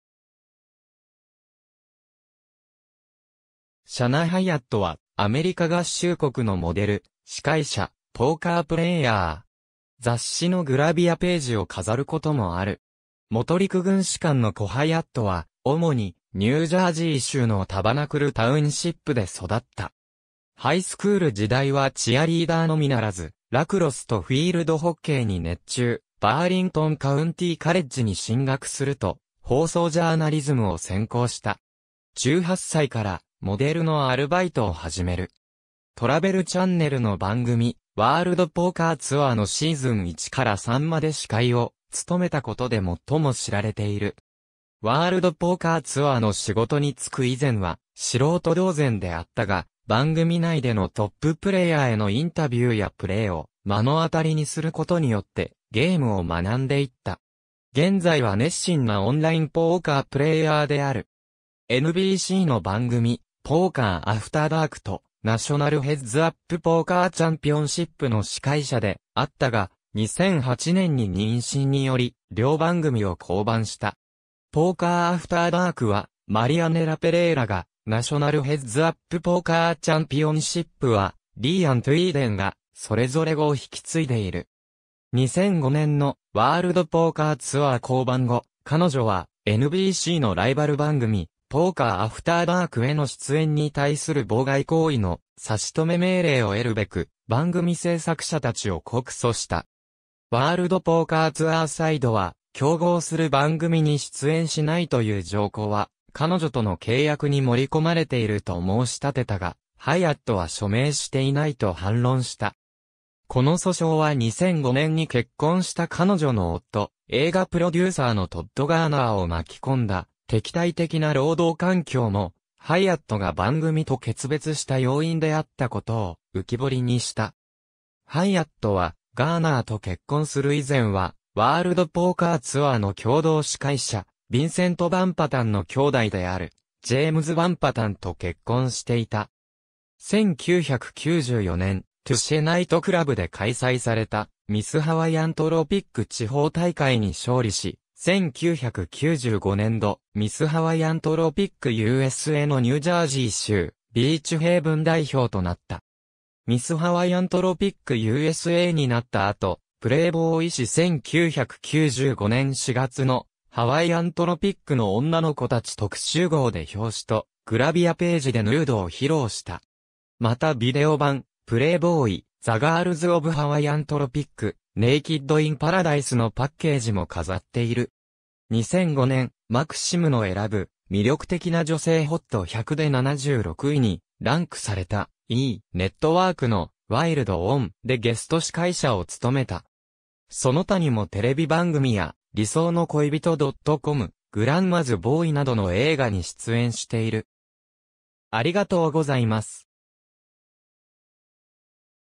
シャナ・ハイアット
シャナ・ハイアット（Shana Hiatt、1975年12月17日 - ）は、アメリカ合衆国のモデル・司会者・ポーカープレイヤー。
雑誌のグラビアページを飾る事もある。
元陸軍士官の子ハイアットは、主にニュージャージー州のタバナクル・タウンシップで育った。
ハイスクール時代はチアリーダーのみならず、ラクロスとフィールドホッケーに熱中、バーリントン・カウンティー・カレッジに進学すると放送ジャーナリズムを専攻した。
18歳からモデルのアルバイトを始める。
トラベル・チャンネルの番組『ワールドポーカーツアー』のシーズン1から3まで司会を務めた事で最も知られている。
『ワールドポーカーツアー』の仕事に就く以前は素人同然であったが、番組内でのトップ・プレイヤーへのインタビューやプレイを目の当たりにする事によってゲームを学んでいった。
現在は熱心なオンラインポーカー・プレイヤーである。
NBCの番組『ポーカー・アフターダーク』と『ナショナル・ヘッズアップ・ポーカー・チャンピオンシップ』の司会者であったが、2008年に妊娠により両番組を降板した。
『ポーカー・アフターダーク』はマリアネラ・ペレイラが、『ナショナル・ヘッズアップ・ポーカー・チャンピオンシップ』はリーアン・トゥイーデンが、それぞれ後を引き継いでいる。
2005年の『ワールドポーカーツアー』降板後、彼女はNBCのライバル番組『ポーカー・アフターダーク』への出演に対する妨害行為の差止命令を得るべく、番組製作者たちを告訴した。
ワールドポーカーツアーサイドは、競合する番組に出演しないという条項は、彼女との契約に盛り込まれていると申し立てたが、ハイアットは署名していないと反論した。
この訴訟は2005年に結婚した彼女の夫、映画プロデューサーのトッド・ガーナーを巻き込んだ「敵対的な労働環境」も、ハイアットが番組と決別した要因であった事を浮彫りにした。
ハイアットはガーナーと結婚する以前は、『ワールドポーカーツアー』の共同司会者ヴィンセント・ヴァン・パタンの兄弟であるジェームズ・ヴァン・パタンと結婚していた。
1994年、トゥシェ・ナイトクラブで開催された「ミス・ハワイアントロピック」地方大会に勝利し、1995年度ミス・ハワイアントロピックUSAのニュージャージー州ビーチヘイブン代表となった。
ミス・ハワイアントロピックUSAになった後、PLAYBOY誌1995年4月の「ハワイアントロピックの女の子たち」特集号で表紙とグラビアページでヌードを披露した。
またビデオ版『Playboy: The Girls of Hawaiian Tropic, Naked in Paradise』のパッケージも飾っている。
2005年、マクシムの選ぶ魅力的な女性HOT100で76位にランクされた E! ネットワークの『Wild On!』でゲスト司会者を務めた。
その他にもテレビ番組や『理想の恋人.com』、『Grandma's Boy』などの映画に出演している